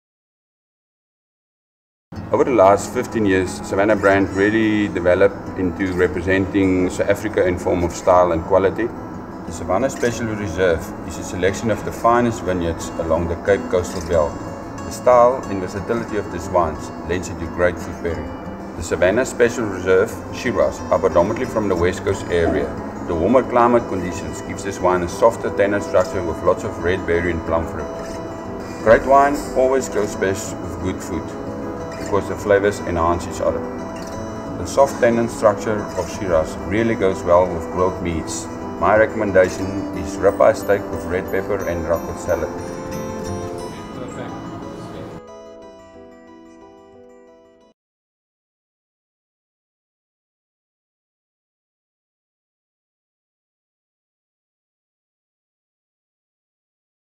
Savanha Special Reserve Shiraz (Danish)
En smuk mørk farve og de forskellige lag af frugtstyret stil med blommer, brombær og antydninger af krydderi. Smagen er rigt komplimenteret af en diskret smag af eg, der fører til en fuldaromatiseret finish med stor frugteftersmag.

Se vores vinmager Frans K Smit præsentere vinsmagningnoter og hans madparringsforslag; Grillet Steak, ruccola og ristet rød peber salat.